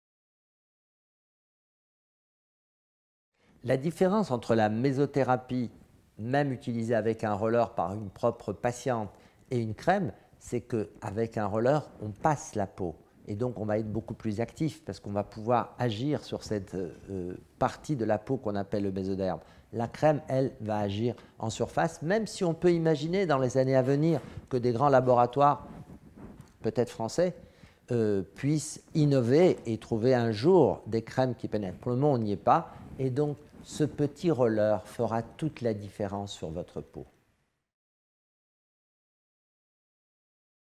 Quelle est la difference majeure entre une crème et un traitement de méso?
La différence entre la mésothérapie même utilisée avec un roller par une patiente,et une simple crème est que avec un roller on passe la peau. Ainsi, le produit sera beaucoup plus actif. L'on va pouvoir mieux agir sur cette partie de la peau que l'on appelle le mésoderme. La crème agit en surface, même si dans les années à venir l'on peut imaginer que des grands laboratoires inventeront des crèmes plus actives, pour le moment ce n'est pas le cas.  Ce roller va donc faire toute la différence sur la peau.